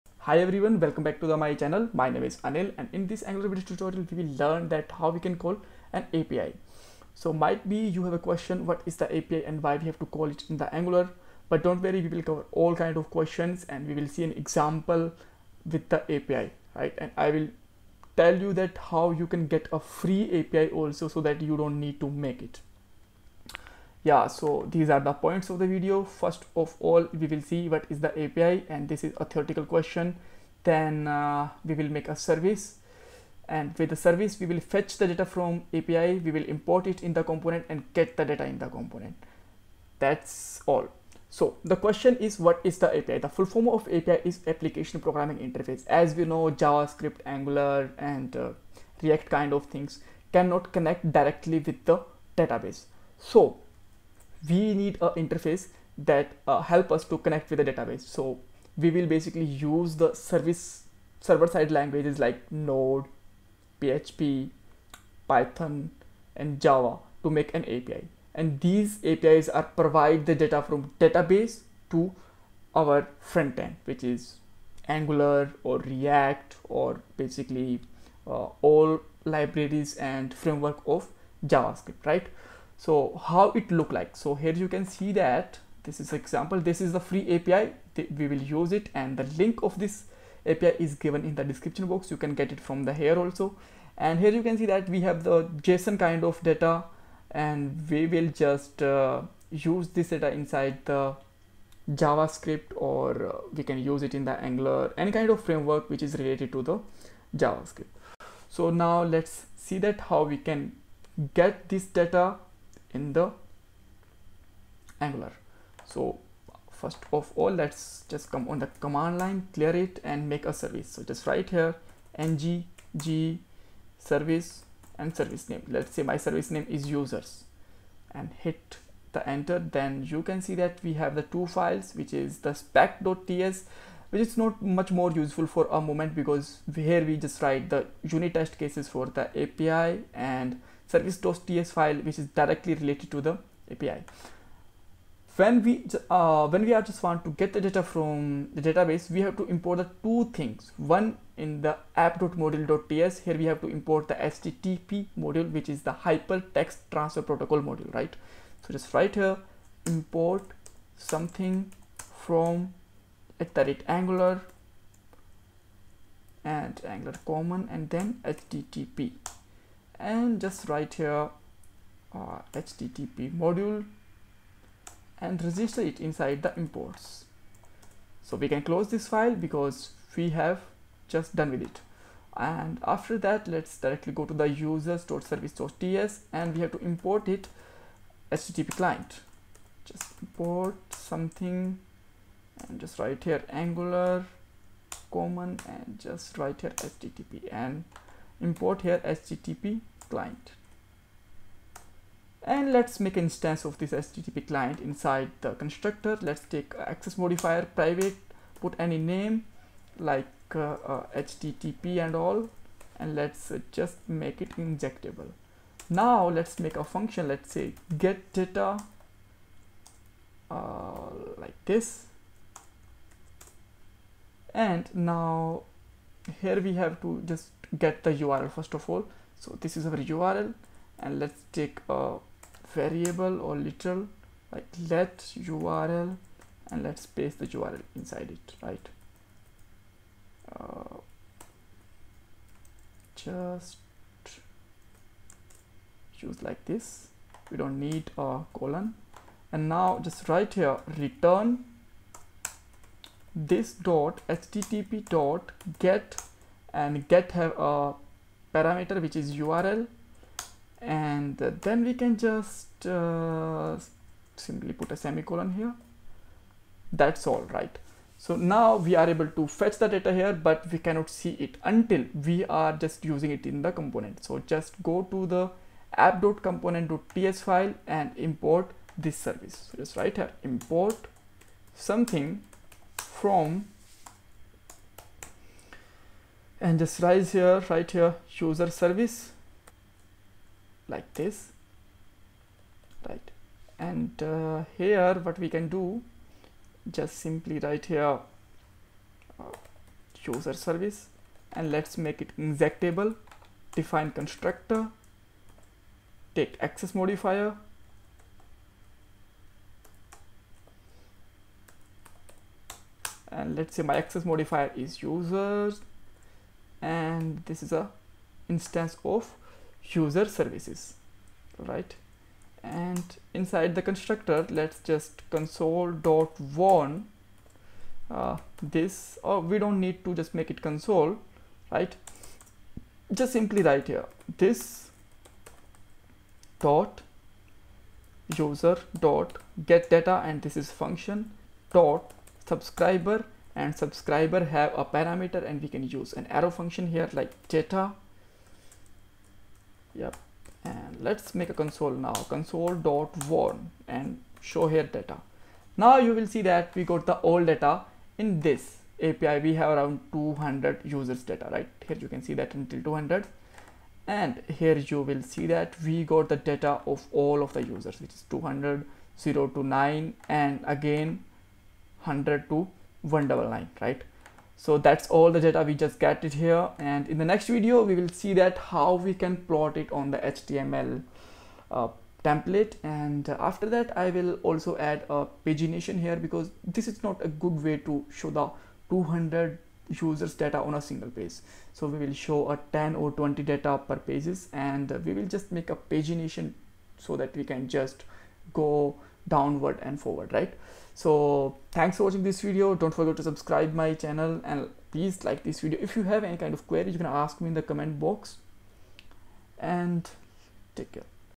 Angular 10 tutorial #28 call API | HTTP
in this Angular 10 and angular 11 tutorial, what is api and how to call api in angular in the English language. This video is made by anil Sidhu

What is service in angular
How to make service
How to use service
Example